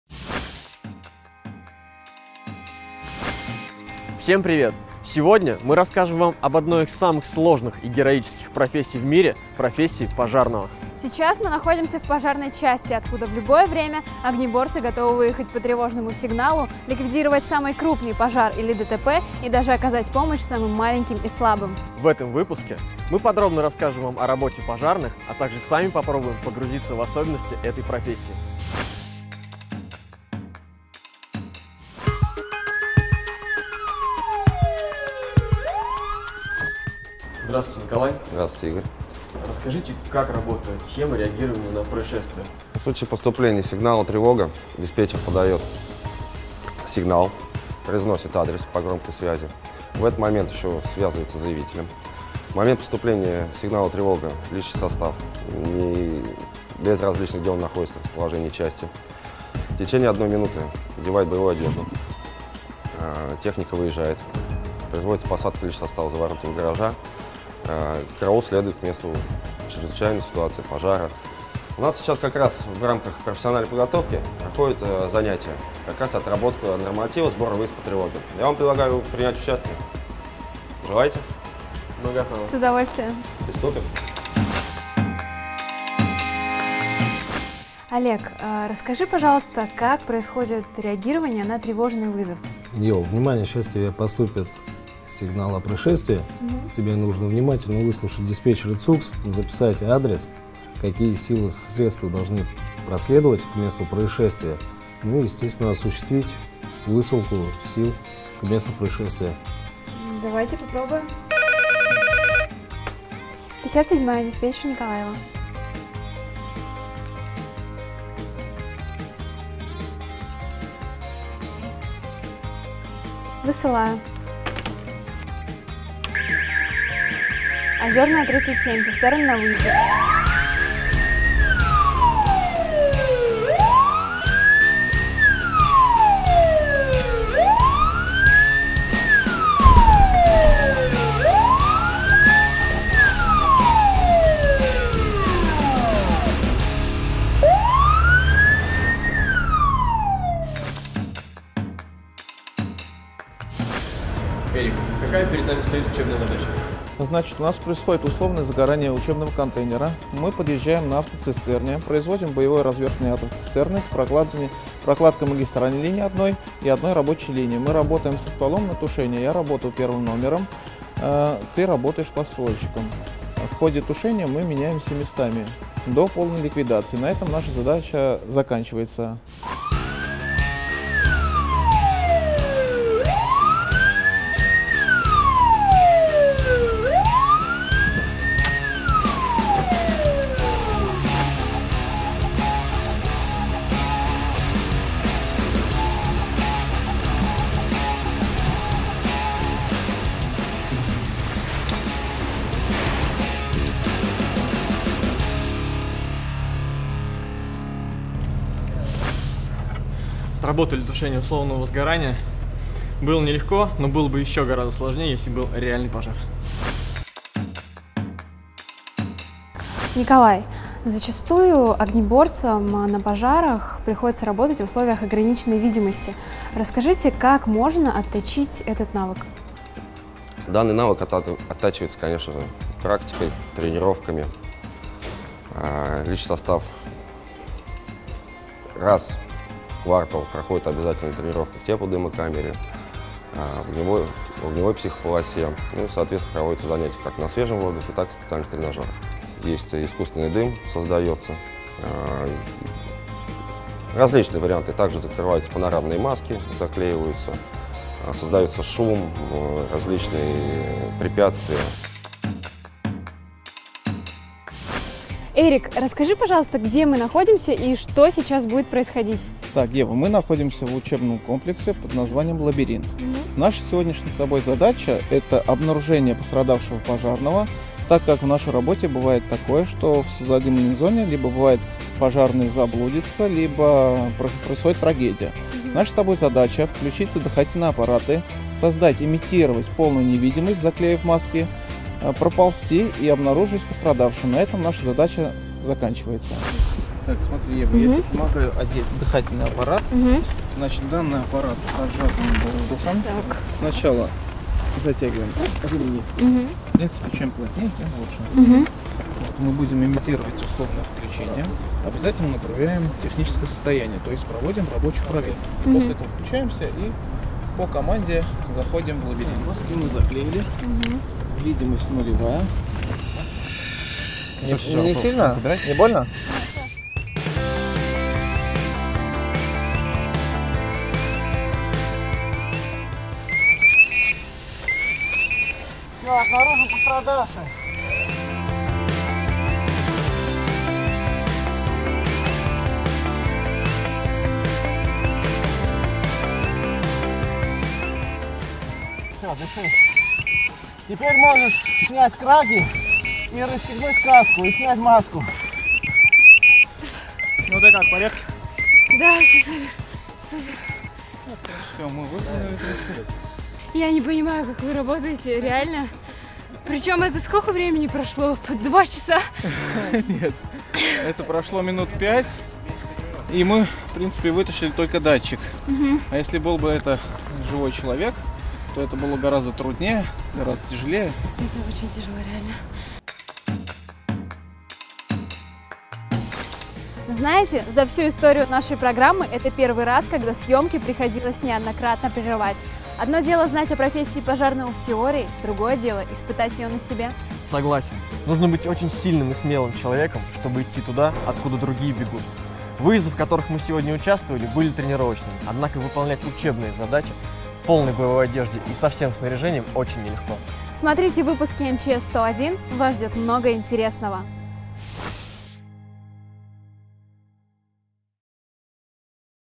ОДИН ДЕНЬ В ПОЖАРНОЙ ЧАСТИ. Тренировка с закрытыми глазами. Развели огонь на территории СПСЧ.
В новом выпуске МЧСВЛОГ Игорь и Ева отправились в специализированную пожарно-спасательную часть по тушению крупных пожаров, чтобы ПОКАЗАТЬ ВАМ ежедневную работу огнеборцев МЧС России.
❓Зачем нужны ТРЕНИРОВКИ С ЗАКРЫТЫМИ ГЛАЗАМИ и для чего пожарные разводят ОГОНЬ НА ТЕРРИТОРИИ СПСЧ - смотрите в новом выпуске!
А еще, прямо во время съемок пожарные ТРИ РАЗА выезжали на реальные происшествия😳 СНИМАТЬ ЭТОТ ВЫПУСК БЫЛО ДЕЙСТВИТЕЛЬНО СЛОЖНО! Поэтому, вы обязательно должны посмотреть его и поделиться с друзьями😉
Впечатления - в комментариях!